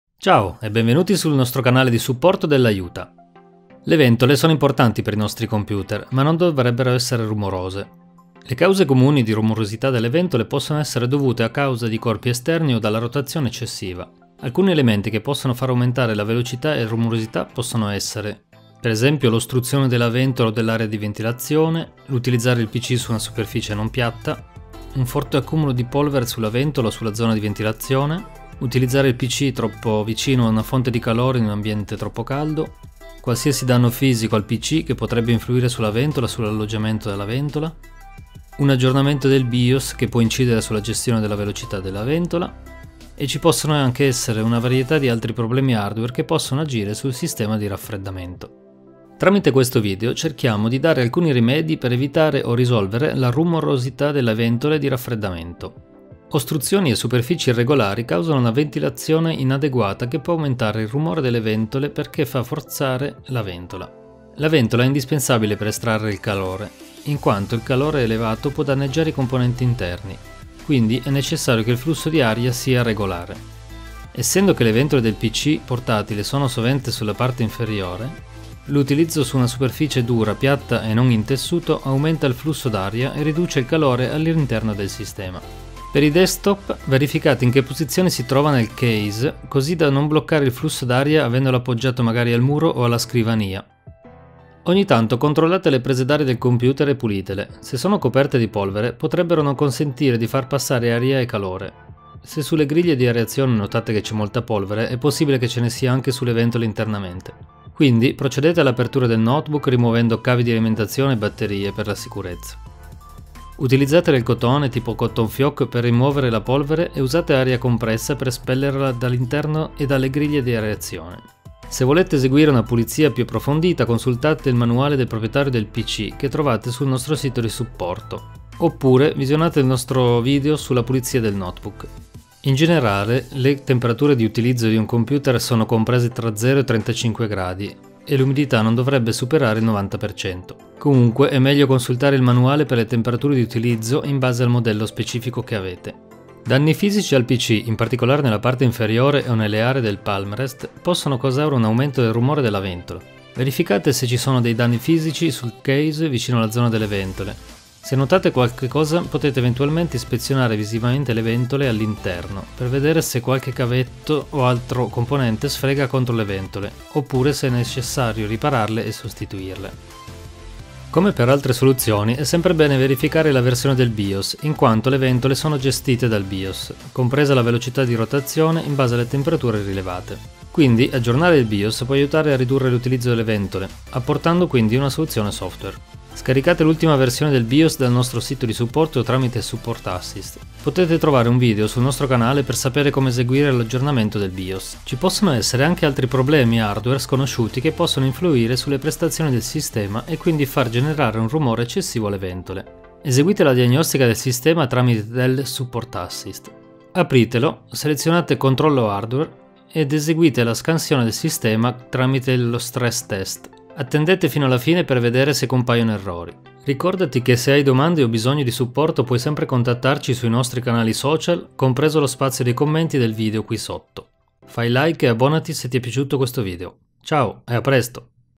Come ridurre la rumorosità delle ventole del computer _ (Supporto Ufficiale Dell)
Le ventole sono importanti per i nostri computer, ma non dovrebbero essere rumorose.
In questo video trattiamo le cause che possono rendere rumorose le ventole e quali rimedi adottare.
 Le cause comuni di rumorosità delle ventole possono essere dovute a causa di corpi esterni o dalla rotazione eccessiva – alcuni elementi che possono far aumentare la velocità e rumorosità possono essere   :

▪ L’ostruzione
▪ Superfici irregolari
▪ Polvere
▪ Calore
▪ Danni
▪ BIOS
▪ Problema hardware